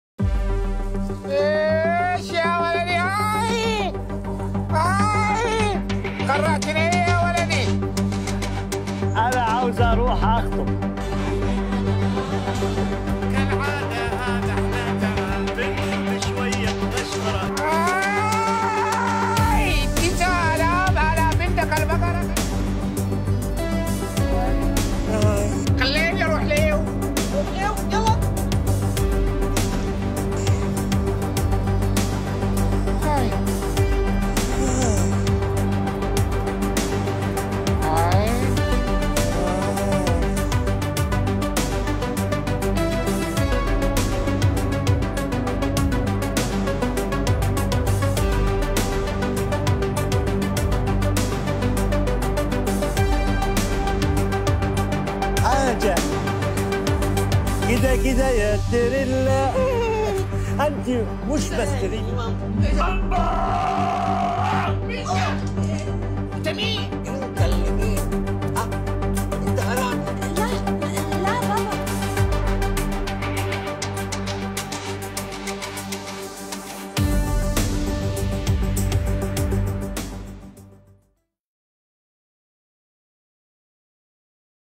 Kuwaiti TV Show Under Fire For Using Blackface
People are calling out this TV show for using blackface to portray Sudanese people